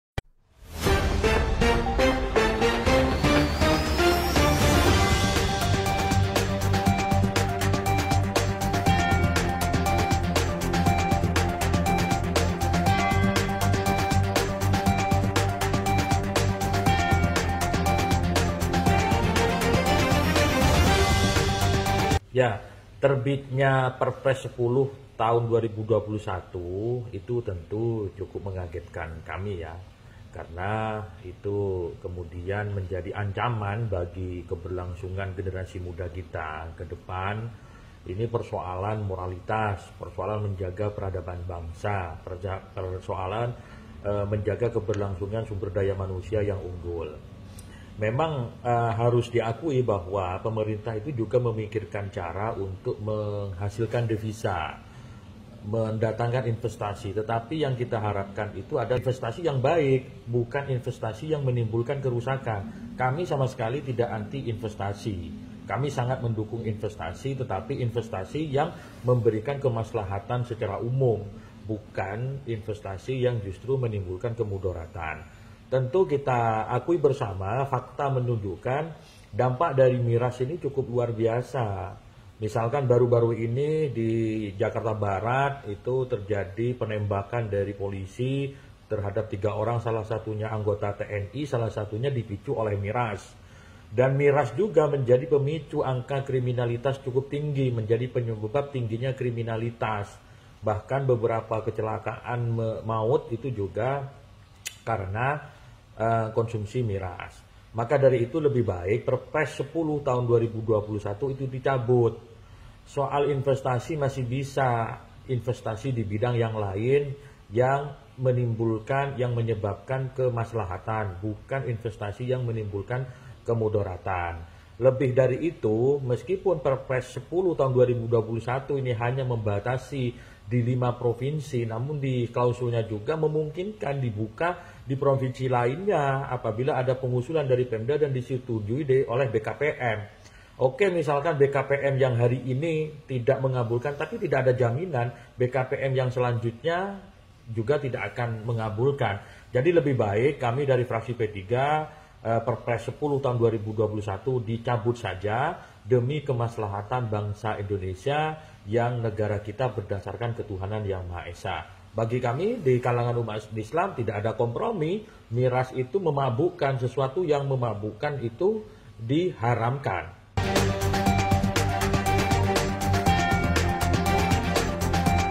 Achmad Baidowi : PPP, CABUT Pengesahan Perpres No. 10 Tahun 2021_MIRAS Pemicu Kerusakan Moral Bangsa
Pengesahan Perpres No. 10 Tahun 2021, yg melegalkan Investasi Minuman Keras di 5 Provinsi. Akan menjadi pemicu kerusakan moral bangsa, dan memicu tindak kriminalitas. Oleh karena itu, PPP dengan Tegas menolak pengesahan Perpres tersebut. Bidang Fungsional DPP PPP, Achmad Baidowi.